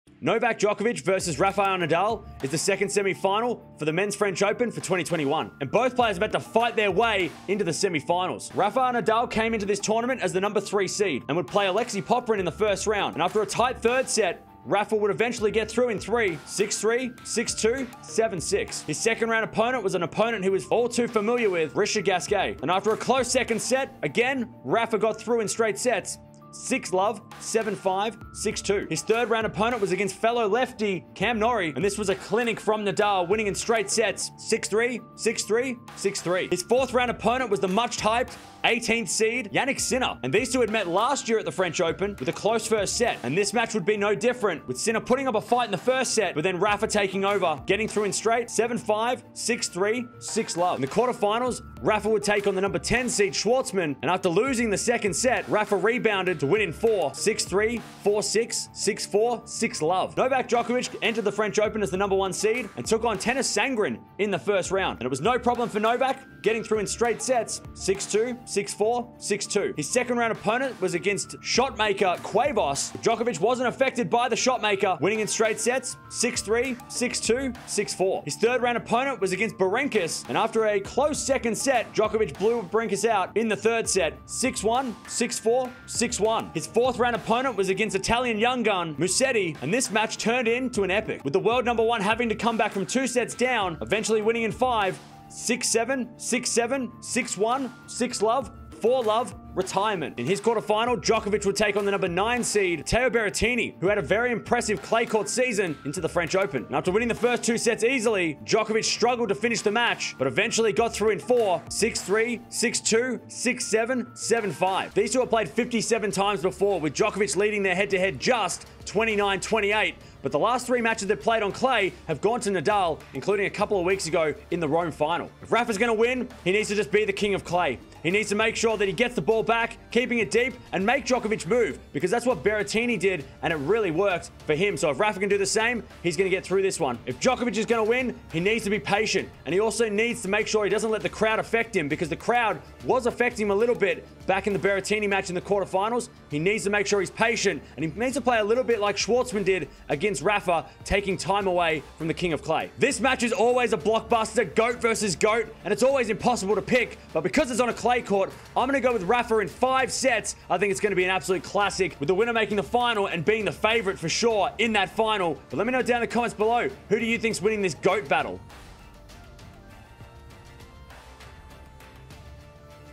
NADAL vs DJOKOVIC | Semi Final Preview | French Open 2021 | Tennis News
Novak Djokovic will take on Rafael Nadal in the Semi Finals of Roland Garros for 2021. It'll be the 58th time these two have faced off in the singles and the second time on the clay courts in 2021 so this will be a fun one! Who will reach the 2021 Final?